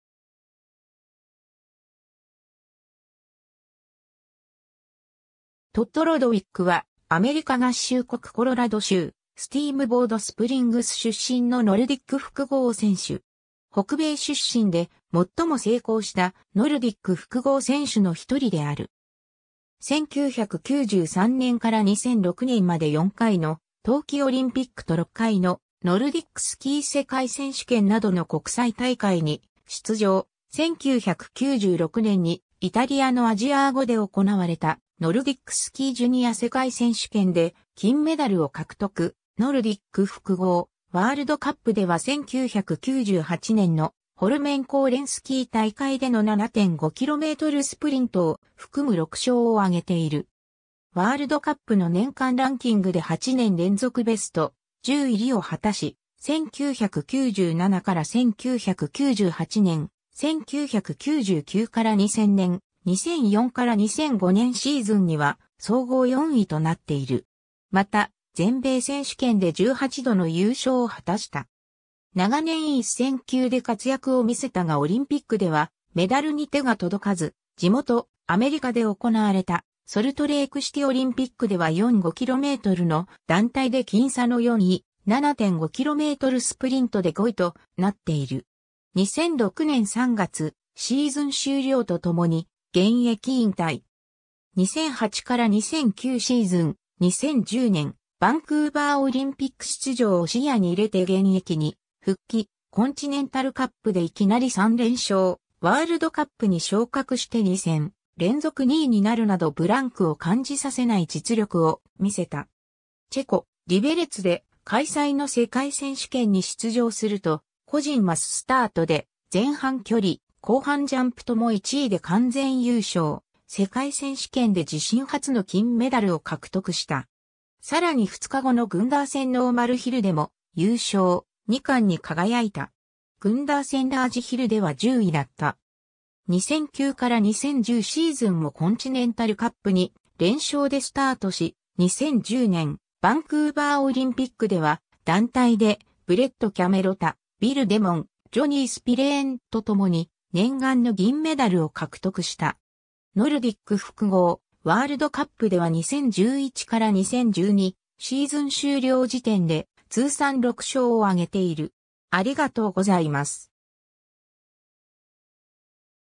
トッド・ロドウィック
トッド・ロドウィック（Todd Lodwick、1976年11月21日 - ）は、アメリカ合衆国コロラド州スティームボードスプリングス出身のノルディック複合選手。
北米出身で最も成功したノルディック複合選手の1人である。
 1993年から2006年まで4回の冬季オリンピックと6回のノルディックスキー世界選手権などの国際大会に出場、1996年にイタリアのアジアーゴで行われたノルディックスキージュニア世界選手権で金メダルを獲得、ノルディック複合・ワールドカップでは1998年のホルメンコーレンスキー大会での7.5kmスプリントを含む6勝をあげている。
ワールドカップの年間ランキングで8年連続ベスト10入りを果たし、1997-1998年、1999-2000年、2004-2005年シーズンには総合4位となっている。
また全米選手権で18度（ノルディック複合で10回、スキージャンプで8回）の優勝を果たした。
 長年一線級で活躍を見せたがオリンピックではメダルに手が届かず、地元アメリカで行われたソルトレークシティオリンピックでは4×5kmの団体で僅差の4位、7.5kmスプリントで5位となっている。
 2006年3月、シーズン終了とともに現役引退。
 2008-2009シーズン、2010年バンクーバーオリンピック出場を視野に入れて現役に復帰、コンチネンタルカップでいきなり3連勝、ワールドカップに昇格して2戦連続2位になるなどブランクを感じさせない実力を見せた。
チェコ、リベレツで開催の世界選手権に出場すると個人マススタートで前半距離、後半ジャンプとも1位で完全優勝、世界選手権で自身初の金メダルを獲得した。
さらに2日後のグンダーセンノーマルヒルでも優勝、2冠に輝いた。
グンダーセンラージヒルでは10位だった。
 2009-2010シーズンもコンチネンタルカップ2連勝でスタートし、2010年バンクーバーオリンピックでは団体でブレット・キャメロタ、ビル・デモン、ジョニー・スピレーンとともに念願の銀メダルを獲得した。
 ノルディック複合・ワールドカップでは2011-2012シーズン終了時点で通算6勝（2位10回、3位12回）をあげている。